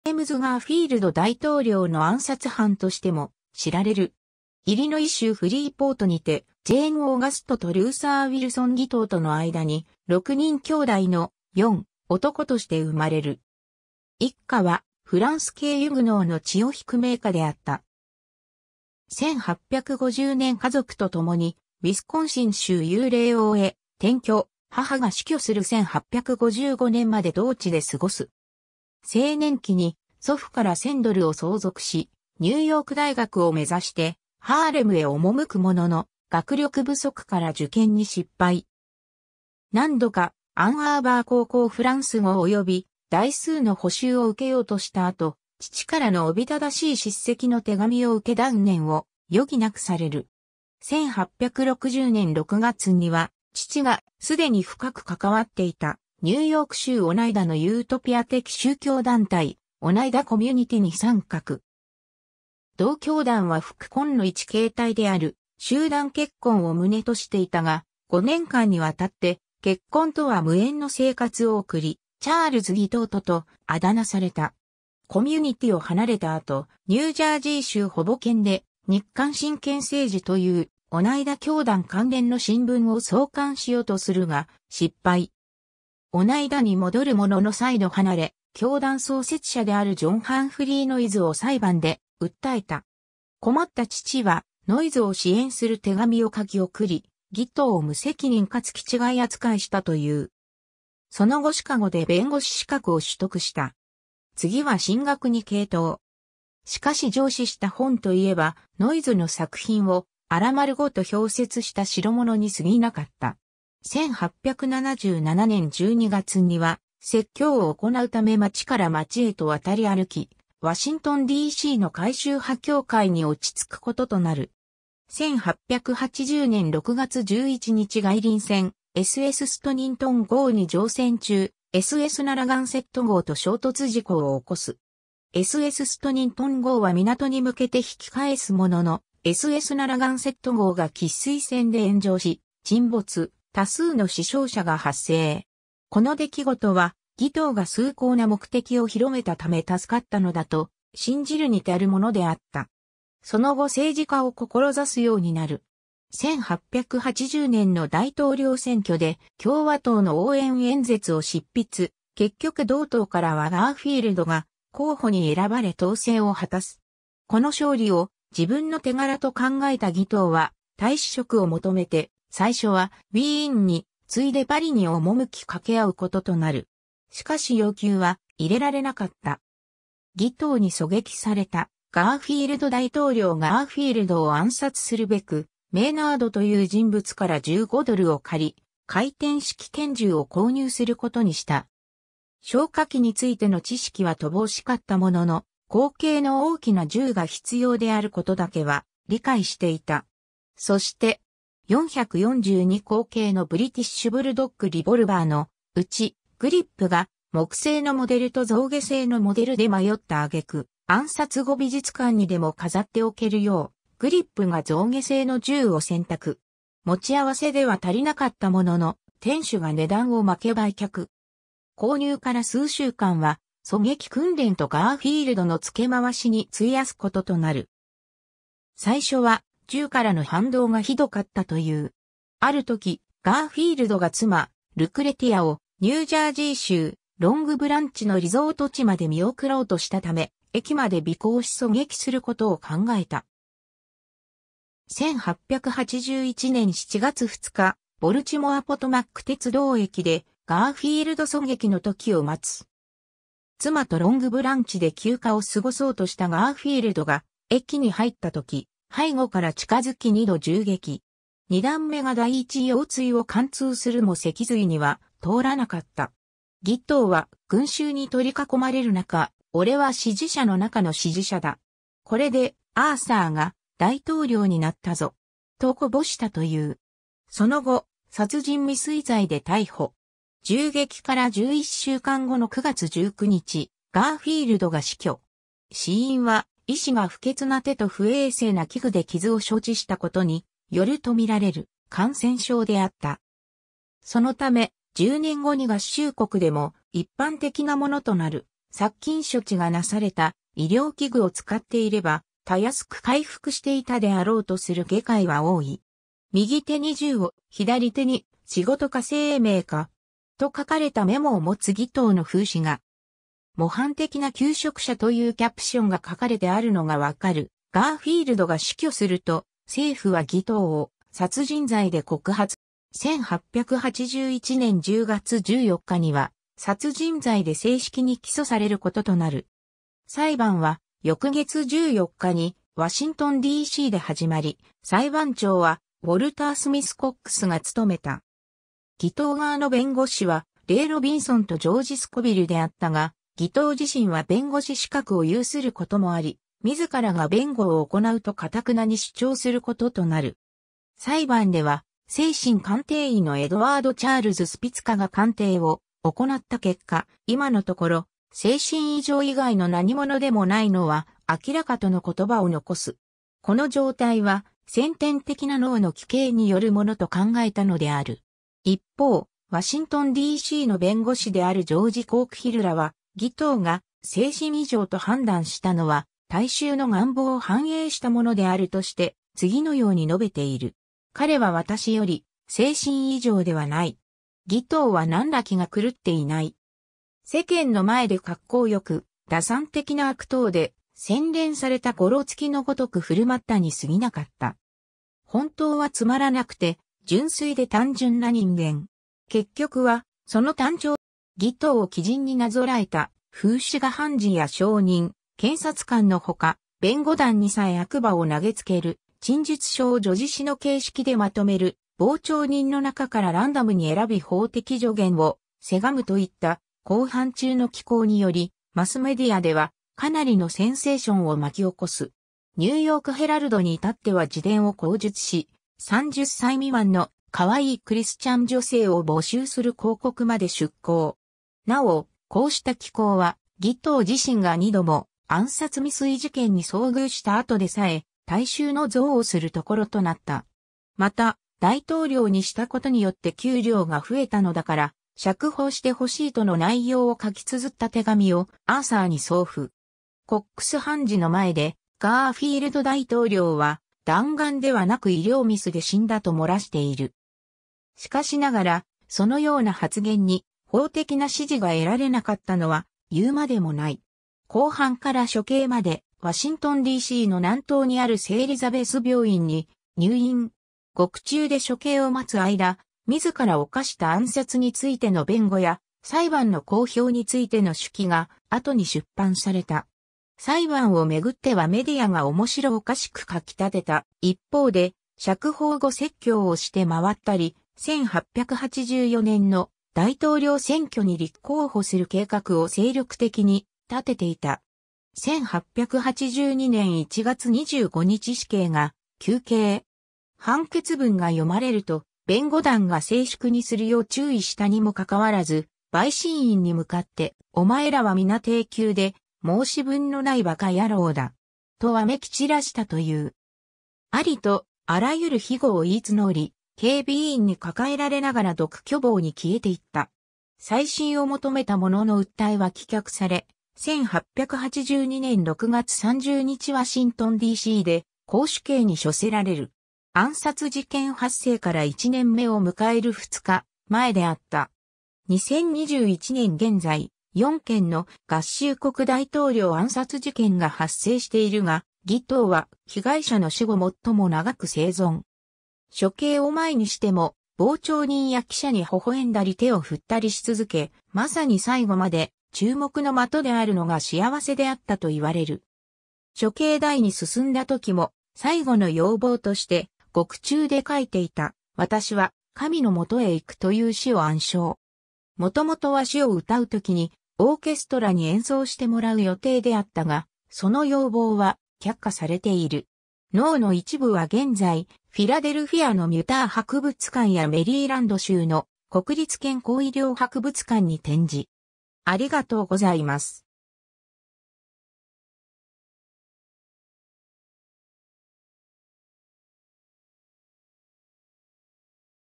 チャールズ・J・ギトー
ジェームズ・ガーフィールド
チャールズ・ジュリアス・ギトー（Charles Julius Guiteau、1841年9月8日 - 1882年6月30日）は、アメリカ合衆国の聖職者、作家、法律家。
ジェームズ・ガーフィールド大統領の暗殺犯としても知られる。
 イリノイ州フリーポートにて、ジェーン・オーガストとルーサー・ウィルソン・ギトーとの間に、6人兄弟の四男として生まれる。
一家はフランス系ユグノーの血を引く名家であった。
1850年家族と共にウィスコンシン州ユーレイオー（現在のグラフトン）へ転居、母が死去する1855年まで同地で過ごす。
 青年期に祖父から1000ドルを相続し、ニューヨーク大学を目指してハーレムへ赴くものの、学力不足から受験に失敗。
何度かアンアーバー高校フランス語及び代数の補習を受けようとした後、父からの夥しい叱責の手紙を受け断念を余儀無くされる。
 1860年6月には、父が既に深く関わっていた、ニューヨーク州オナイダのユートピア的宗教団体 オナイダ・コミュニティに参画。
同教団は複婚の一形態である集団結婚を旨としていたが、5年間にわたって結婚とは無縁の生活を送り、「チャールズ・ギトート(Gitout、 Guiteauに"get out"を掛けたもの)」と渾名された。
 コミュニティを離れた後、ニュージャージー州ホボケンで「日刊神権政治」という、オナイダ教団関連の新聞を創刊しようとするが失敗。
オナイダに戻るものの再度離れ、教団創設者であるジョン・ハンフリー・ノイズを裁判で訴えた。
困った父は、ノイズを支援する手紙を書き送り、ギトーを無責任かつ気違い扱いしたという。
その後シカゴで弁護士資格を取得した。
 次は神学に傾倒。
しかし上梓した本と言えば、ノイズの作品を粗丸ごと剽窃した代物に過ぎなかった。
1877年12月には説教を行うため町から町へと渡り歩き、ワシントンD.C.の会衆派教会に落ち着く事となる。
 1880年6月11日外輪船SSストニントン号に乗船中、SSナラガンセット号と衝突事故を起こす。
SSストニントン号は港に向けて引き返すものの、SSナラガンセット号が喫水線で炎上し沈没、多数の死傷者が発生（SSストニントン号の乗客で負傷者はおらず）。
この出来事は、ギトーが崇高な目的を広めたため助かったのだと信じるに足るものであった。
 その後政治家を志すようになる。
1880年の大統領選挙で共和党の応援演説を執筆、結局同党からはガーフィールドが候補に選ばれ当選を果たす。
この勝利を自分の手柄と考えたギトーは、大使職を求めて最初はウィーンに、次いでパリに赴き掛け合う事となる。
しかし要求は容れられなかった。
 ギトーに狙撃されたガーフィールド大統領（傍らにいるのはジェイムズ・G・ブレイン国務長官） ガーフィールドを暗殺するべく、メイナードという人物から15ドルを借り、回転式拳銃を購入する事にした。
小火器についての知識は乏しかったものの、口径の大きな銃が必要である事だけは理解していた。
そして.442口径のブリティッシュ・ブルドッグ・リボルバーのうち、グリップが木製のモデルと象牙製のモデルで迷った挙句、暗殺後美術館にでも飾っておけるよう、グリップが象牙製の銃を選択。
 持ち合わせでは足りなかったものの、店主が値段を負け売却（犯行に使われた銃は回収され、20世紀初頭にはスミソニアン財団が写真撮影さえしたものの、その後行方不明）。
購入から数週間は、狙撃訓練とガーフィールドの付け回しに費やす事となる。
最初は銃からの反動が酷かったという。
 ある時、ガーフィールドが妻ルクレティアをニュージャージー州ロングブランチのリゾート地まで見送ろうとしたため、駅まで尾行し狙撃する事を考えた。
1881年7月2日、ボルチモア・ポトマック鉄道駅（現存せず）でガーフィールド狙撃の時を待つ。
妻とロングブランチで休暇を過ごそうとしたガーフィールドが駅に入った時、背後から近付き2度銃撃。
2弾目が第1腰椎を貫通するも脊髄には通らなかった。
ギトーは群衆に取り囲まれる中、「俺は支持者の中の支持者だ。これでアーサーが大統領になったぞ!」と零したという。
その後、殺人未遂罪で逮捕。
 銃撃から11週間後の9月19日、ガーフィールドが死去。
死因は...